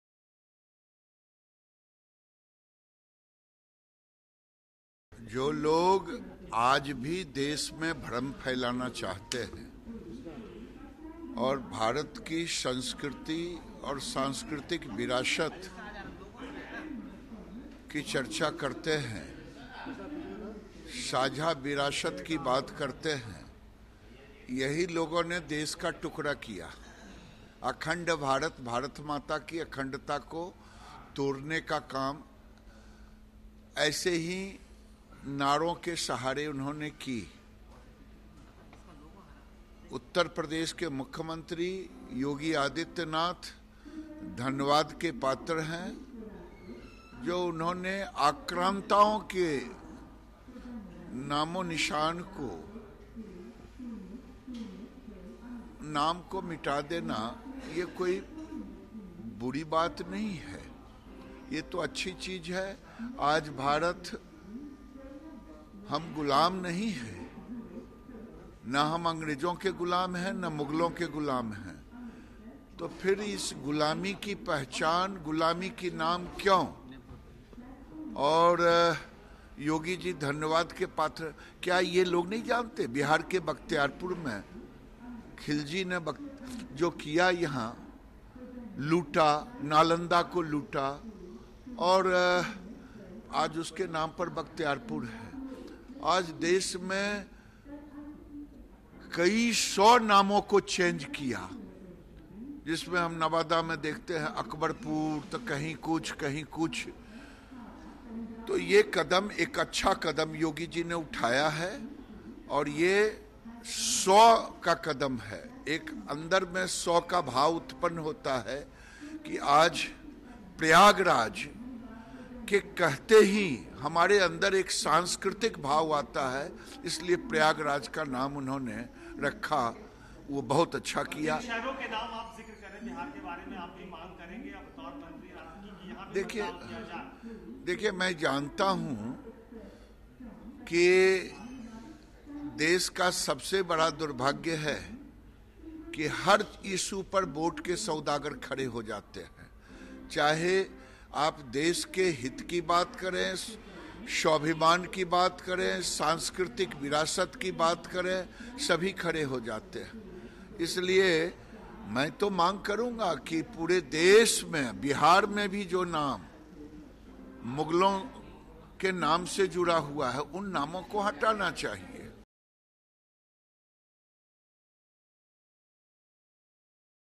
Change names of places associated with Mughals: Giriraj Singh - Bihat News
Patna, Oct 22 (ANI):  Minister of State for Micro, Small and Medium Enterprises, Giriraj Singh on Monday called for change of all names that are associated with Mughals. He appreciated Uttar Pradesh Chief Minister Yogi Adityanath's initiative in this direction.